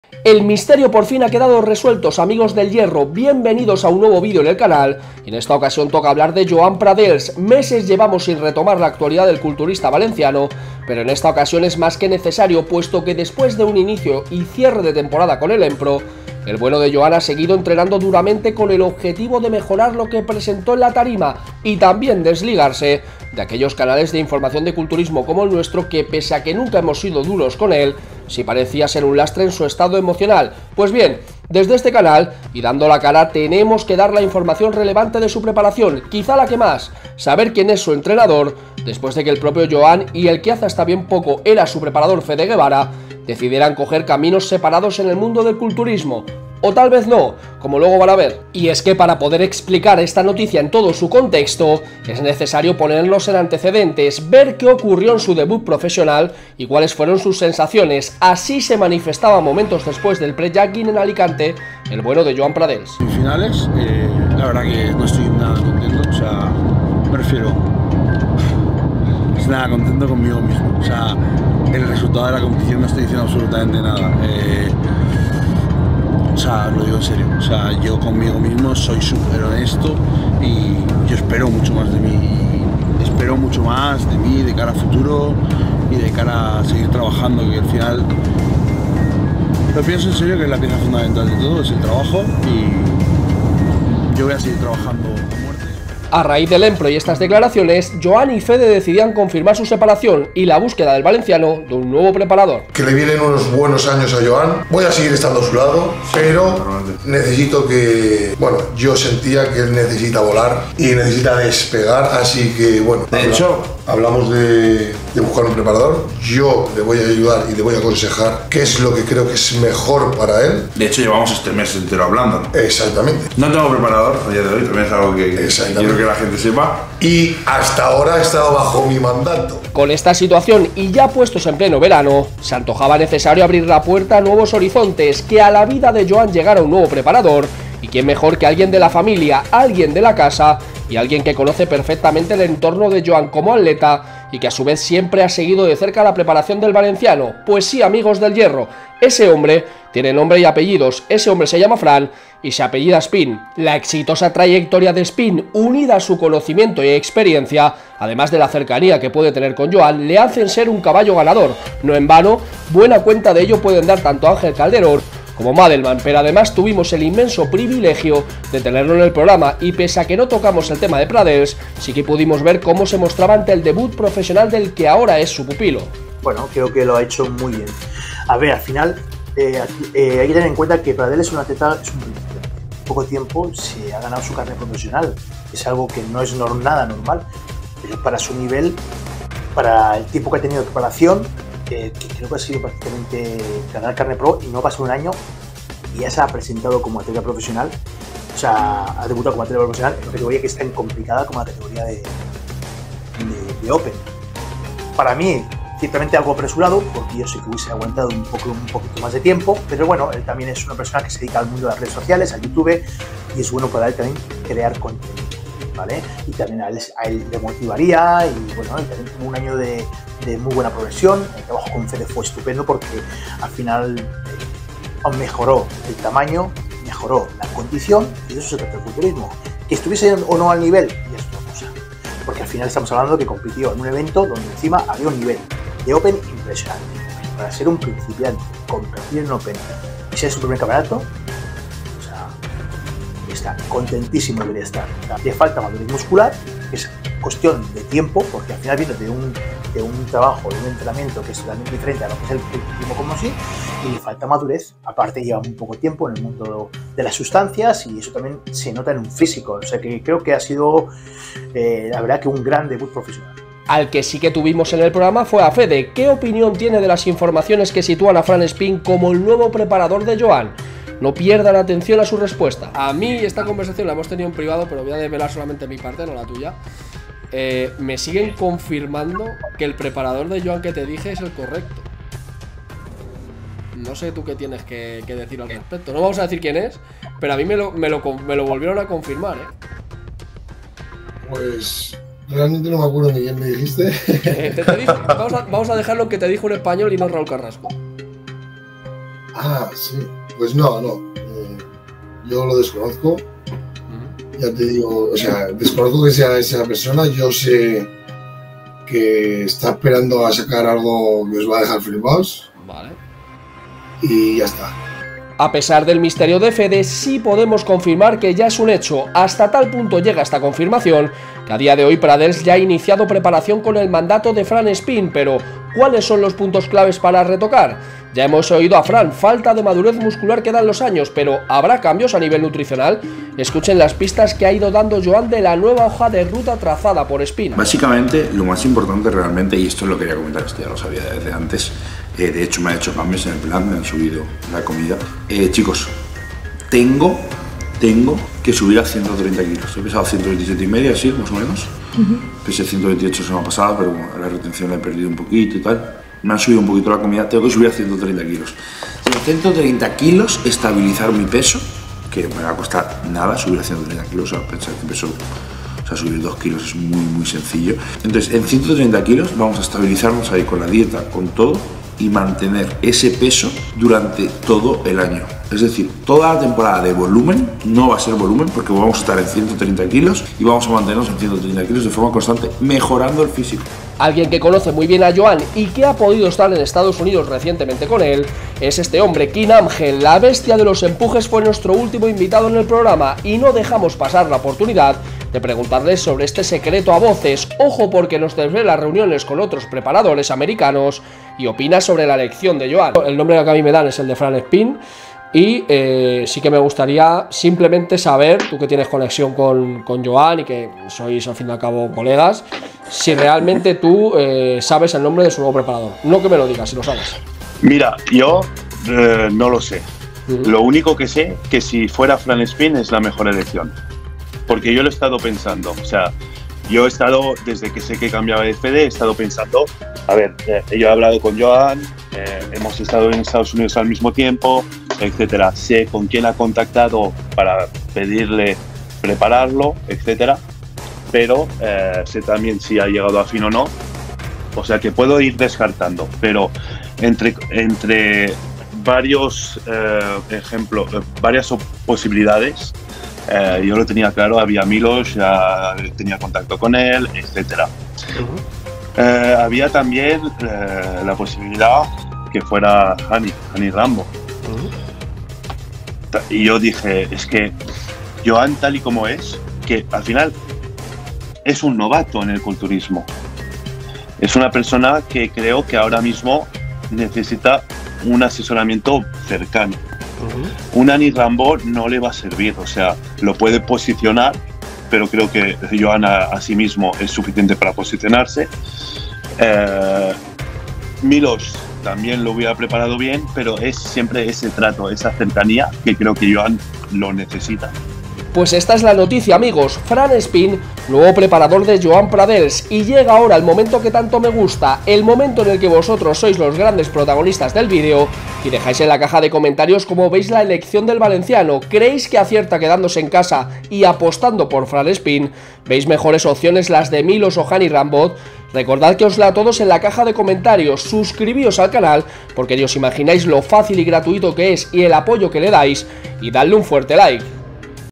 EXCLUSIVA| ¡CONFIRMAMOS el NUEVO preparador de Joan Pradells! ¡Así lo ven las ESTRELLAS del fitness!
¡Éste es el nombre del nuevo preparador de Joan Pradells! El atleta valenciano ha cerrado el círculo y por fin ya tiene la nueva hoja de ruta de su nuevo entrenador para iniciar la preparación.
¿Acertará? Kim Angel y su antiguo preparador Fede Guevara se mojan sobre la elección que marcará la carrera del valenciano en el mundo del culturismo.